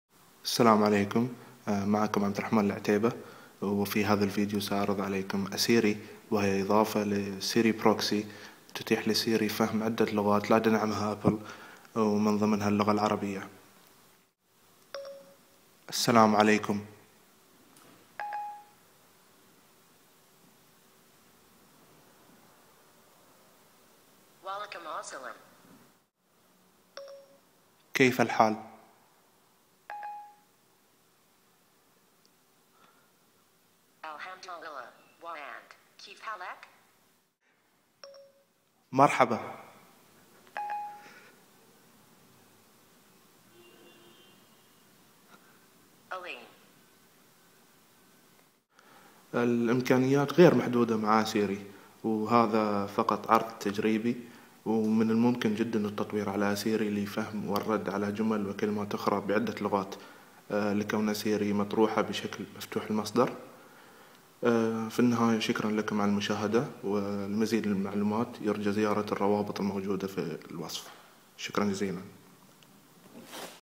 سيري تفهم وتتحدث العربية Siri Talks and Understands Arabic
دع سيري تتحدث العربية مع أسيري، إضافة للسيري بروكسي تتيح إضافة لغات عديدة لسيري. حيث أنها تستخدم خدمة جوجل للتعرف على الصوت. يمكن التحكم بخيارات أسيري للتحدث بالكثير من اللغات المدعومة من جوجل.

Let Siri speak your language with Asiri, the SiriProxy plugin that adds multi language support to Siri. Thanks to Google speech recognition service, Asiri can be configured to speak any language that is supported by Google.